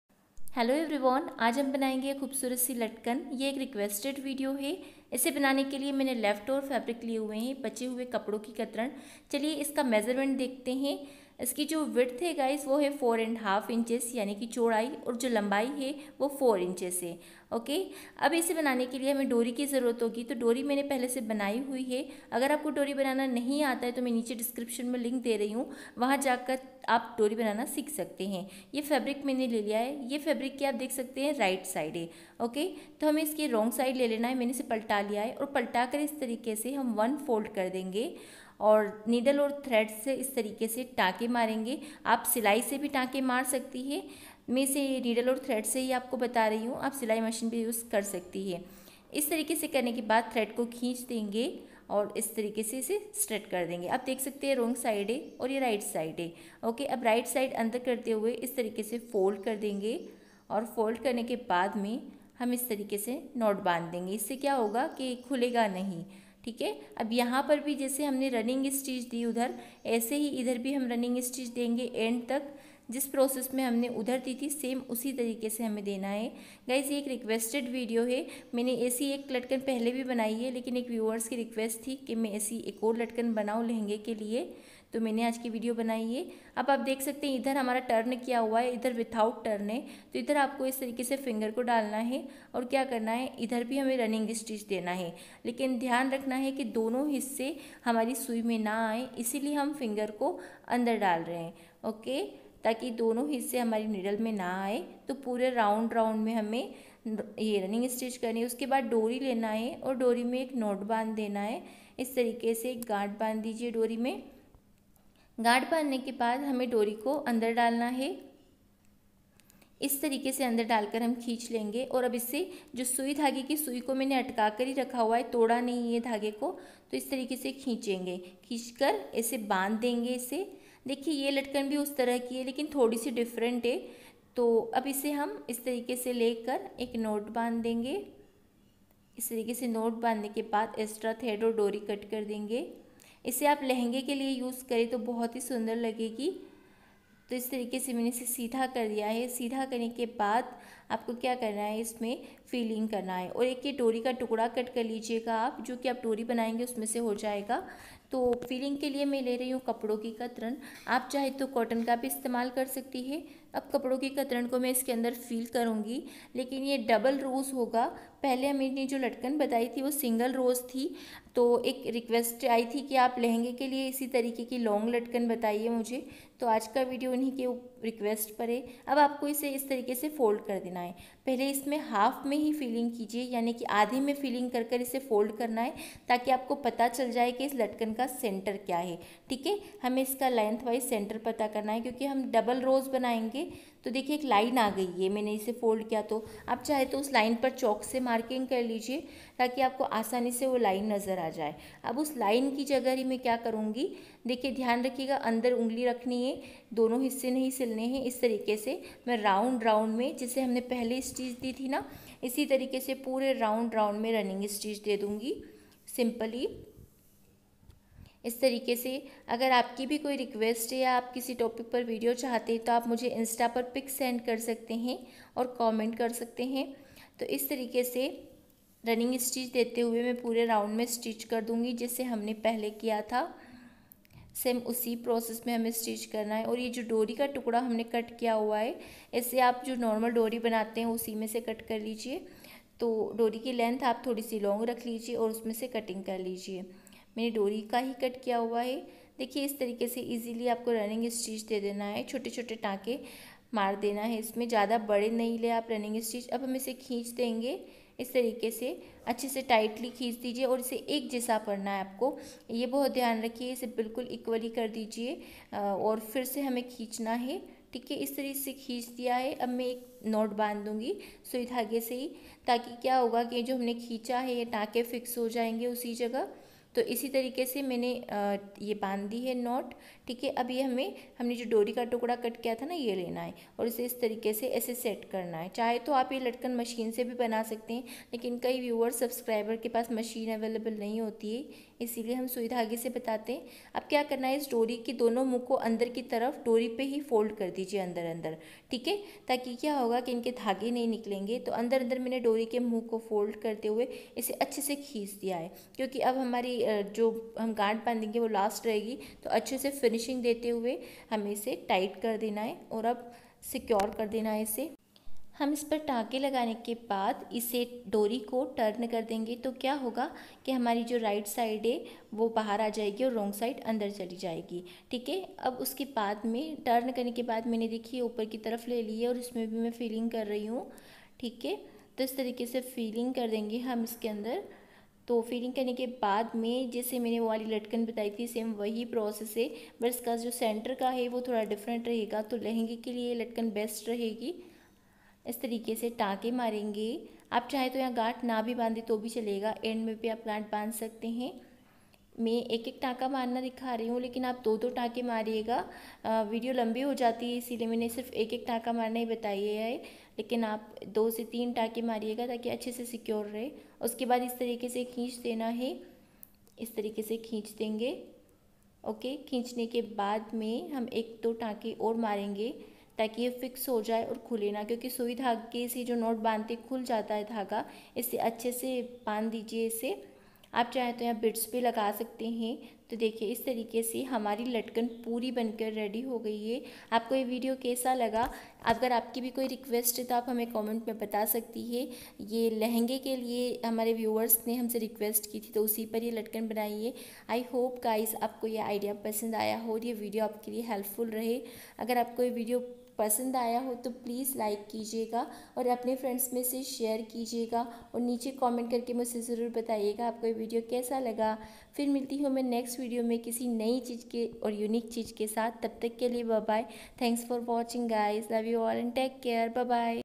👌कपड़ों की कतरन से बनाएं गुलाब के फूल जैसी लटकन सिर्फ 2 मिनट में//how to make latkan for lehnga...
लटकन के बिल्कुल नए डिजाइंस बनाना सीखने के लिए नीचे दी गई लिंक पर क्लिक करें 👇👇